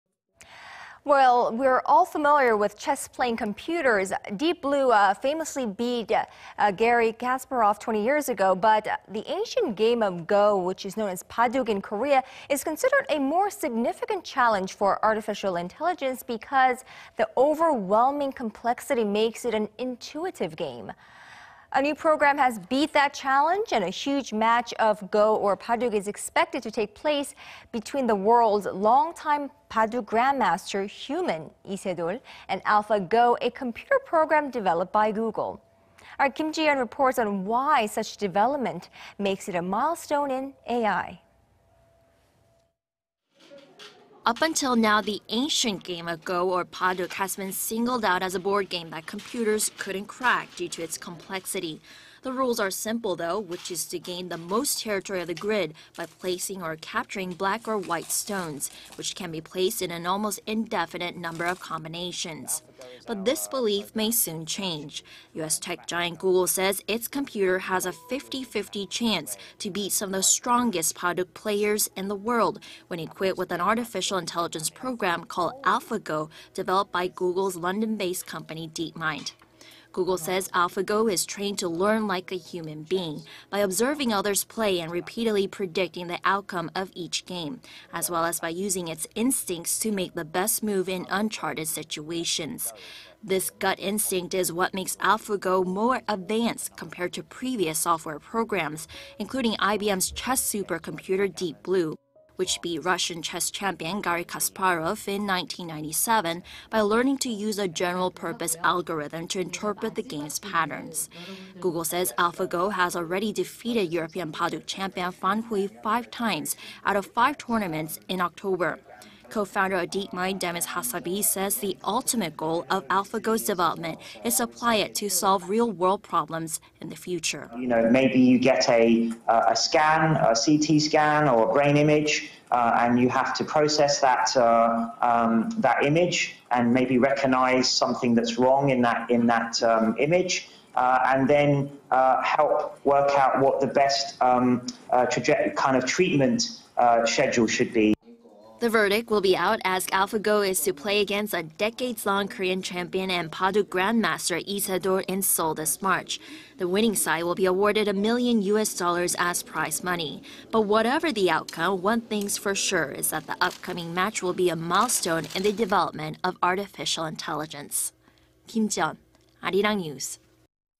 Google's AI program to compete against Korean champion Lee Sedol
이세돌, 이번엔 현존 최강 '바둑 인공지능'과 자존심 대결
We’re all familiar with chess-playing computers - Deep Blue famously beat Garry Kasparov 20 years ago.
But, the ancient game of Go, known as Baduk in Korea is considered a more significant challenge for Artificial Intelligence because the overwhelming complexity makes it an "intuitive" game.
Well, a new program has beat that challenge... and a huge match of Go is expected to take place between the world's long-time Baduk grandmaster Lee Se-dol and AlphaGO, a computer program developed by Google.
Kim Ji-yeon reports on why such development makes it a milestone in AI.
Up until now, the ancient game of Go or Baduk has been singled out as a board game that computers couldn't crack due to its complexity.
The rules are simple though, which is to gain the most territory of the grid by placing or capturing black or white stones... which can be placed in an almost indefinite number of combinations.
But this belief may soon change.
U.S. tech giant Google says its computer has a 50-50 chance to beat some of the strongest Baduk players in the world... when equipped with an Artificial Intelligence program called AlphaGO, developed by Google's London-based company DeepMind.
Google says AlphaGO is trained to learn like a human being... by observing others play and repeatedly predicting the outcome of each game... as well as by using its "instincts" to make the best move in uncharted situations.
This gut instinct is what makes AlphaGO more advanced compared to previous software programs including IBM's chess supercomputer Deep Blue, which beat Russian chess champion Garry Kasparov in 1997. by learning to use a general-purpose algorithm to interpret the game's patterns.
Google says AlphaGO has already defeated European Baduk champion Fan Hui, five times out of five tournaments in October.
Co-founder of DeepMind, Demis Hassabis says the ultimate goal of AlphaGO's development is to apply it to solve real world problems in the future.

"After getting a CT scan, or brain image and you have it to process that image and recognize something what's wrong in that image and help work out what the best kind of treatment schedule should be."

The verdict will be out... as AlphaGO is to play against a decades-long Korean champion and Baduk grandmaster Lee Se-dol in Seoul this March.
The winning side will be rewarded a million U.S. dollars as prize money... but whatever the outcome,... one thing's for sure is that the upcoming match will be a milestone in the development of Artificial Intelligence.
Kim Jiyeon, Arirang News.